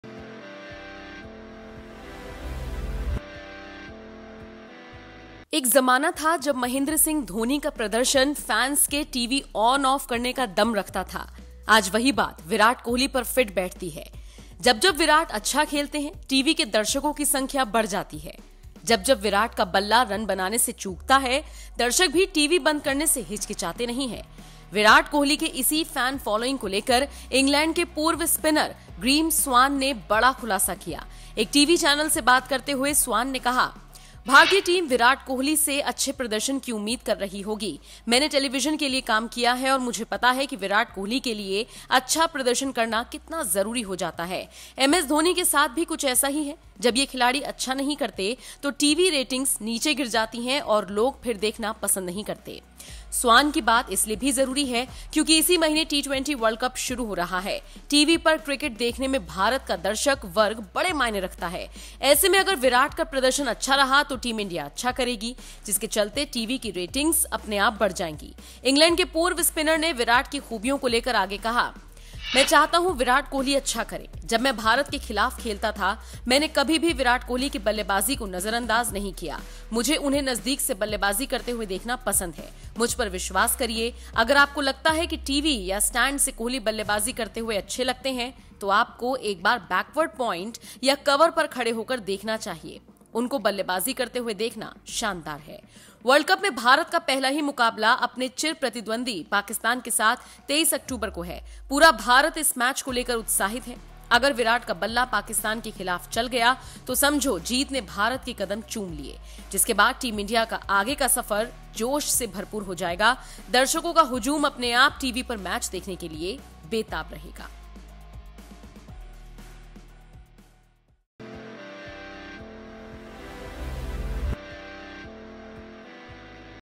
Virat Kohli पर बड़ा खुलासा, T-20 वर्ल्ड कप का हिट फ्लॉप होना Virat के हाथों में है
ग्रीम स्वान ने बताया है कि भारत के स्टार बल्लेबाज विराट कोहली की अच्छी या खराब फॉर्म का असर टीवी रेटिंग पर पड़ता है। वह चाहते हैं कि विराट कोहली आगामी मेगा इवेंट में अच्छा प्रदर्शन करें।